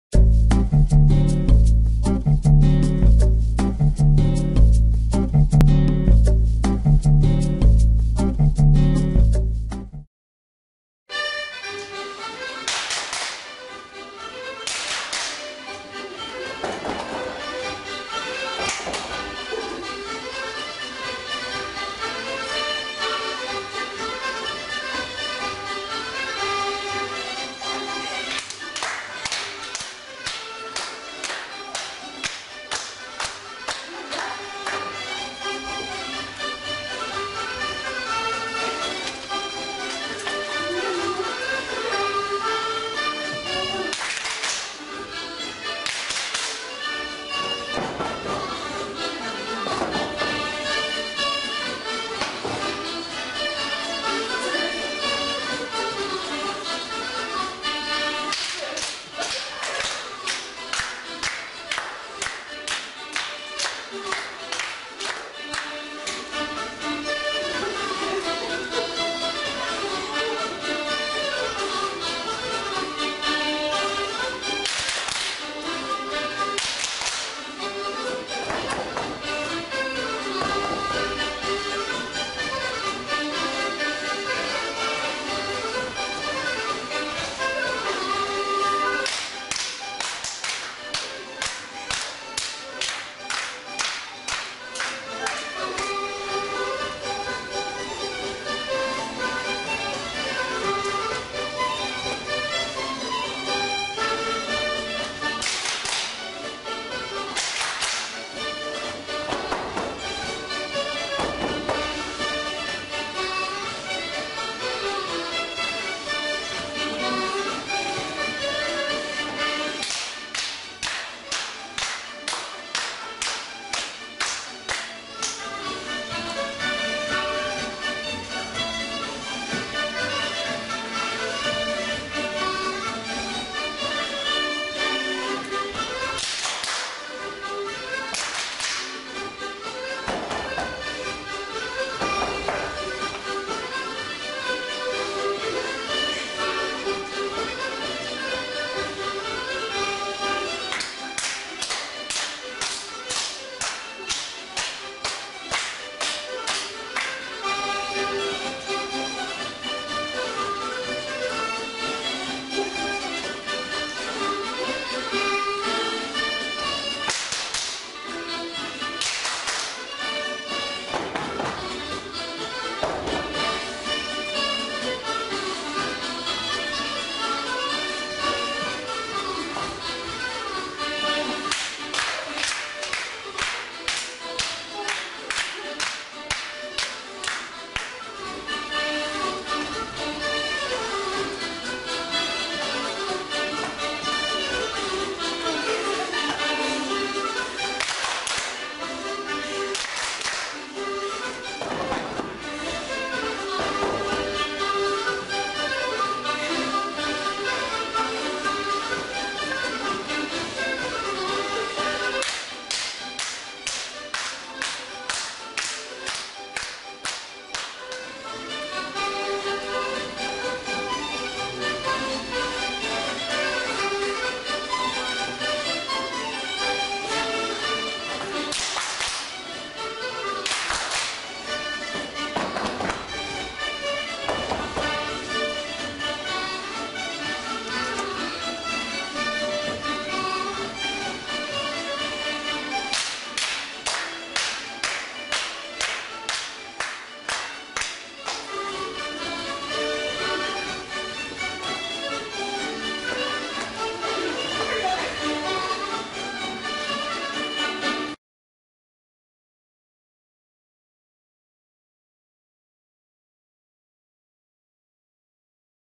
Taniec szkocki Black Dance (Reel)
Taniec w wykonaniu członkiń Koła Naukowego Glottodydaktyki Language Cracker oraz Wykładowców WSF w ramach projektu Kultura Szkocji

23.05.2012, Wyższa Szkoła Filologiczna we Wrocławiu